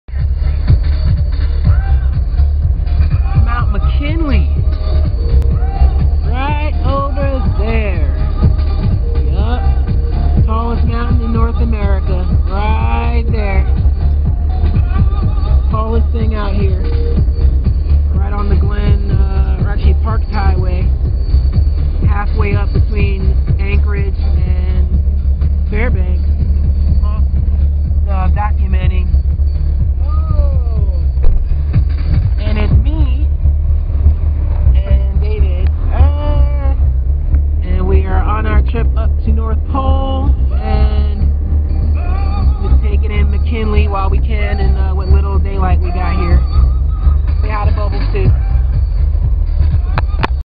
Christmas 2010 North Pole AK and Delta Junction - CLIP0009.AVI
quick clip shot on the Parks Hwy approaching Denali on our drive up to North Pole, AK!